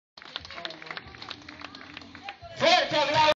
LEONES MARINOS EN EL ZOO TERRA NATURA MURCIA 020
Magnifica y muy bonita exhibición de estos magníficos ejemplares de leones marinos del zoológico de Murcia llamado Terra Natura. Si te gustan los animales ve este vídeo, muy bonito.